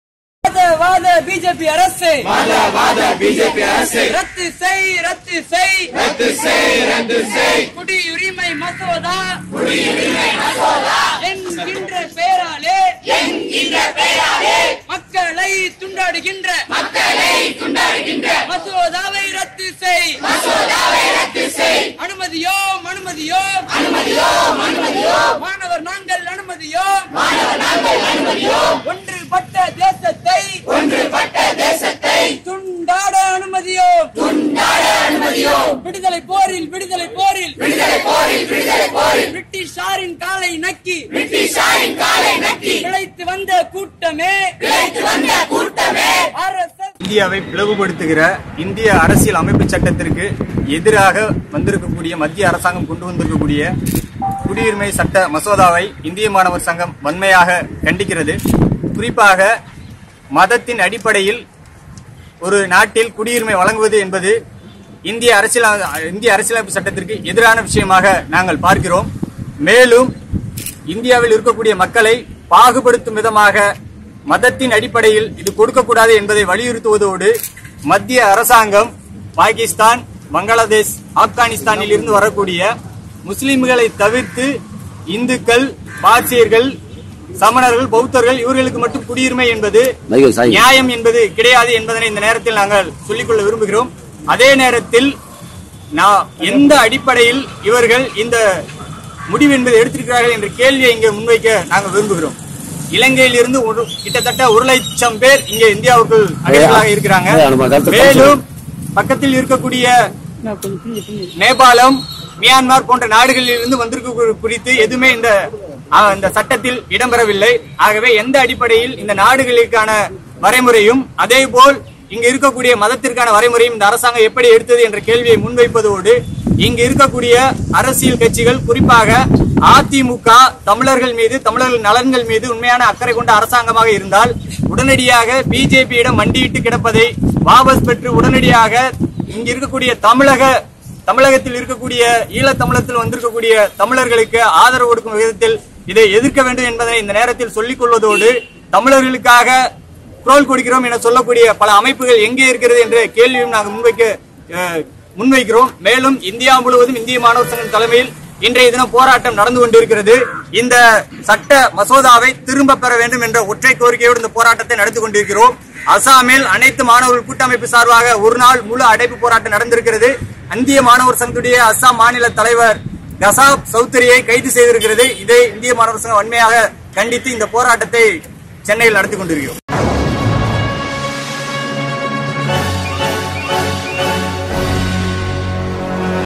குடியுரிமை சட்டத்திருத்த மசோதா நகல் எரிப்பு | SFI | Stuggle | CAB Bill
குடியுரிமை திருத்த மசோதாவை (CAB) ரத்து செய்யக் கோரியும், தேசிய குடிமக்கள் பதிவேட்டை திரும்பப் பெற வேண்டுமென்றும் இந்திய மாணவர் சங்கம் தேசம் முழுவதும் நடத்தும் நகல் எரிப்பு போராட்டத்தின் பகுதியாக சென்னை பல்கலைக்கழகத்தில் இந்திய மாணவர் சங்கம் சார்பில் நடைபெற்றது.